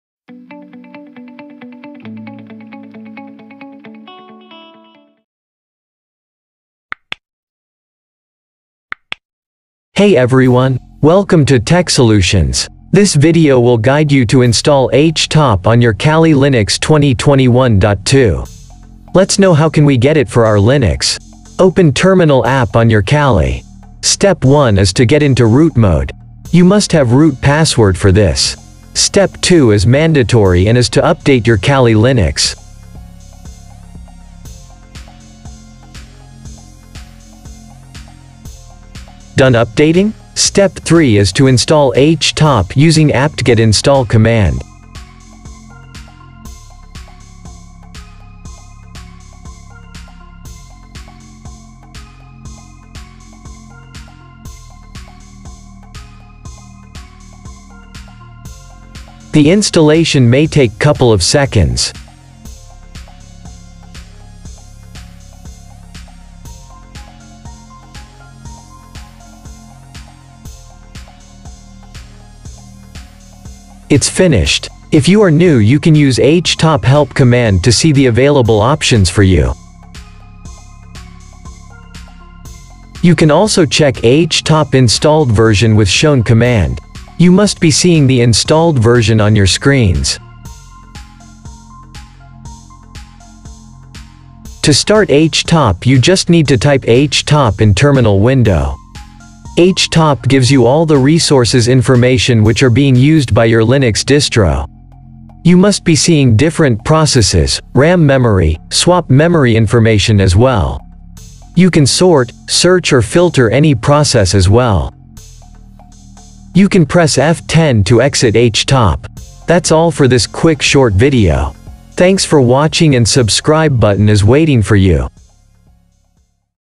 How to Install HTop on Kali Linux 2021.2 | Linux Resource Monitor Tool | Linux Task Manager HTop
This Video will guide you how to install HTop on Linux. HTop is the best Resource Monitor Tool in Kali Linux 2021.2. HTop is designed as an alternative of the Unix program top which is by default tool in Unix or Linux distro. HTop shows all updated lists of the processes running on a computer.